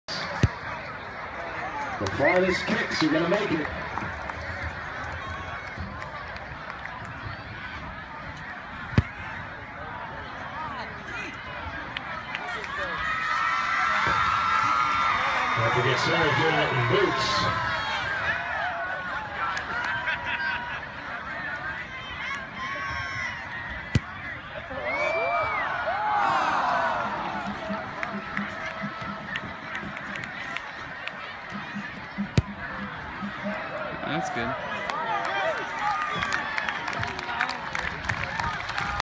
Kicking For Cash 2015 - Vandegrift High School Part 5 - 3rd Place
After 1st & 2nd place were determined, the remaining kickers attempt kicks for 3rd place.  Curran Kelly made the 35-yard field goal for third place.

Kickers:  Lizzie Patterson (Girls LaCrosse), Bryan Nguyen (Choir), Sarah Tabash (Legacies), and Curren Kelly (NHS).